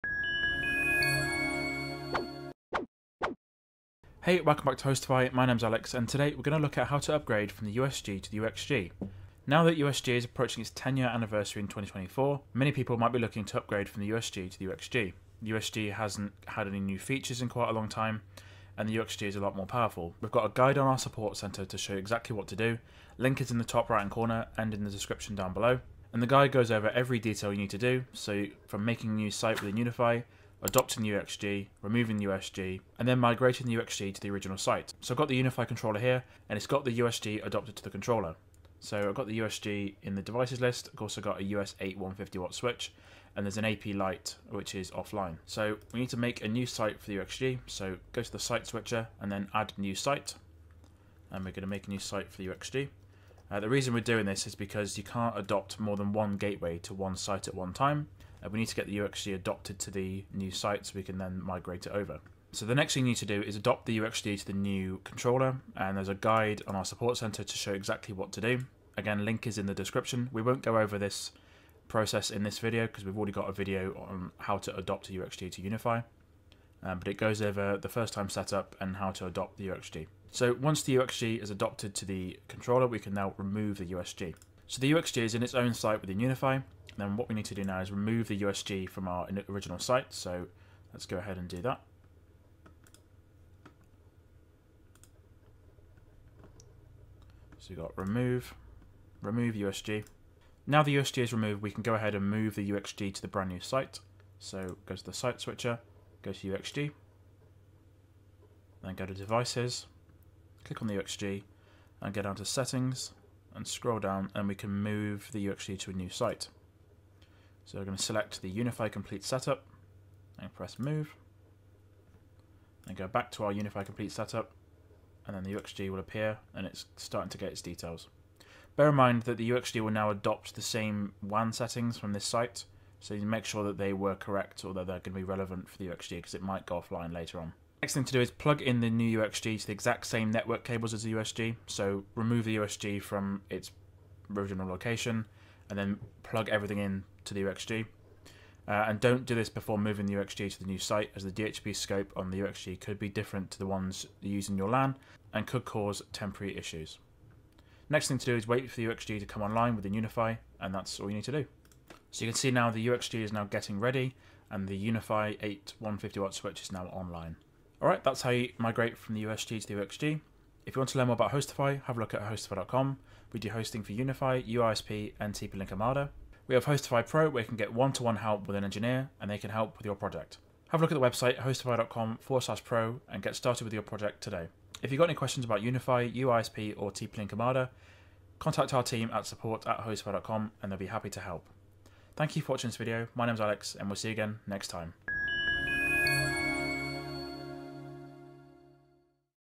USG to UXG Migration Guide
In this guide, we're going to look at how to migrate from the older USG devices (USG 3P and USG Pro 4) to the newer line of UXG devices.